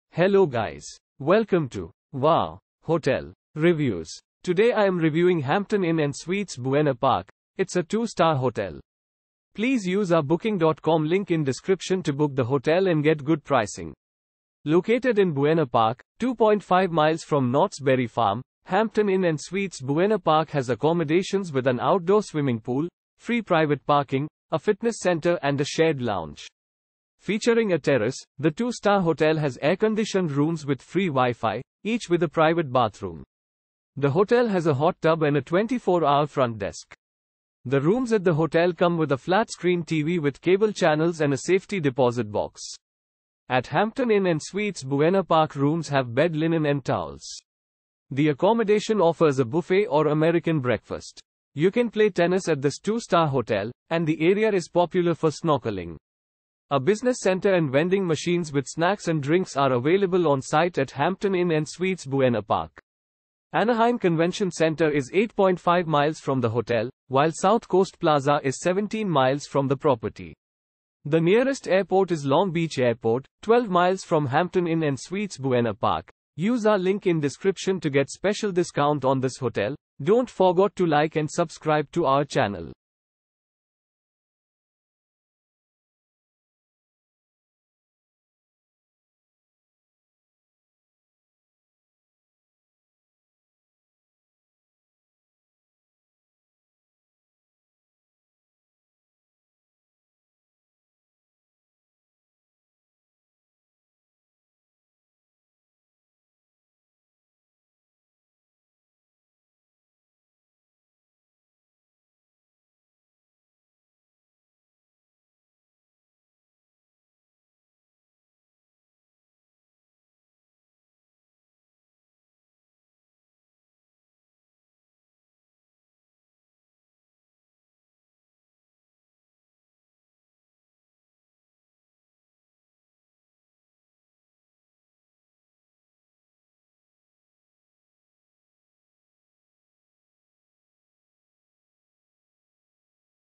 Hampton Inn & Suites Buena Park Review - Buena Park , United States of America
Hampton Inn & Suites Buena Park

Located in Buena Park, 2.5 miles from Knotts Berry Farm, Hampton Inn & Suites Buena Park has accommodations with an outdoor swimming pool, free private parking, a fitness center and a shared lounge. Featuring a terrace, the 2-star hotel has air-conditioned rooms with free WiFi, each with a private bathroom. The hotel has a hot tub and a 24-hour front desk.
The rooms at the hotel come with a flat-screen TV with cable channels and a safety deposit box. At Hampton Inn & Suites Buena Park rooms have bed linen and towels.
The accommodation offers a buffet or American breakfast.
You can play tennis at this 2-star hotel, and the area is popular for snorkeling.
A business center and vending machines with snacks and drinks are available on site at Hampton Inn & Suites Buena Park.
Anaheim Convention Center is 8.5 miles from the hotel, while South Coast Plaza is 17 miles from the property. The nearest airport is Long Beach Airport, 12 miles from Hampton Inn & Suites Buena Park.